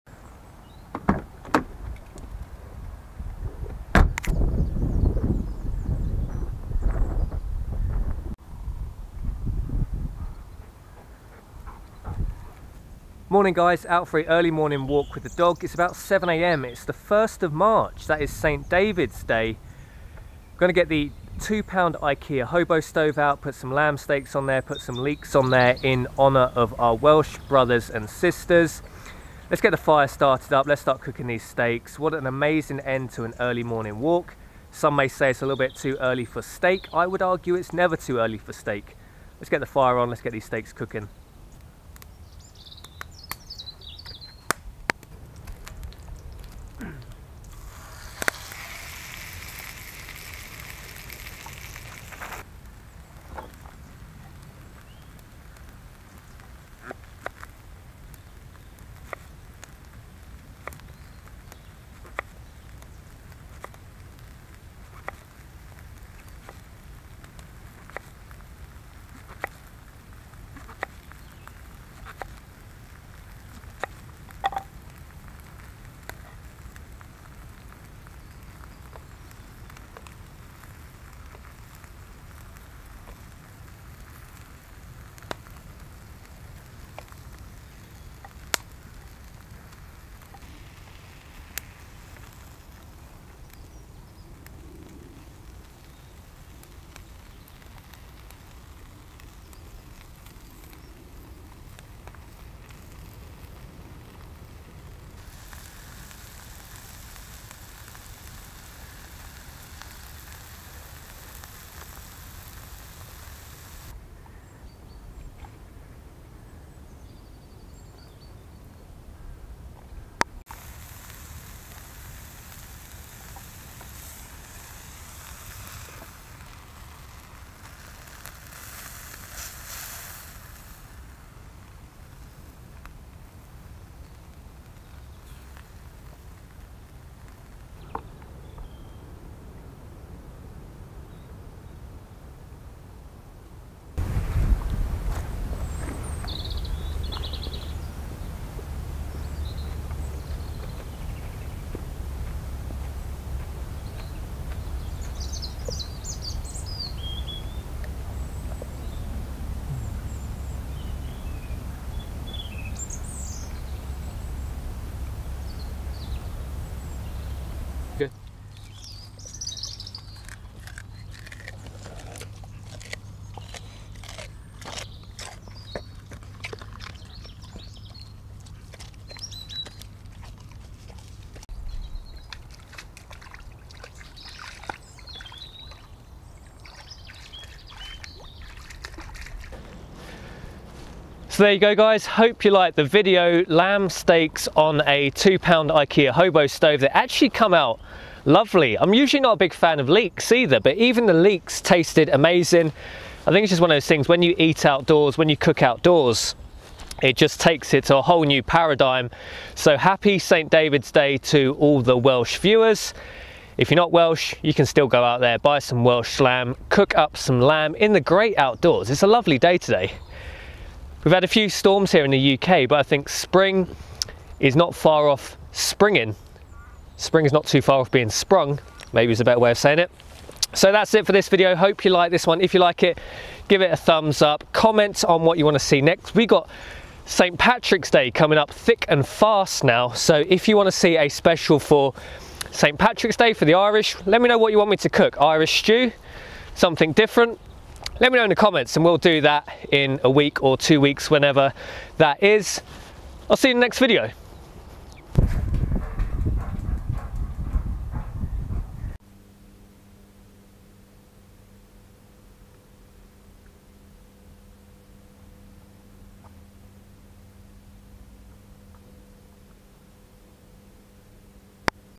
Lamb and Leek on a Wood Stove - St David's Day Cooking
Cooking lamb steak and leek to celebrate St. David the national patron saint of Wales.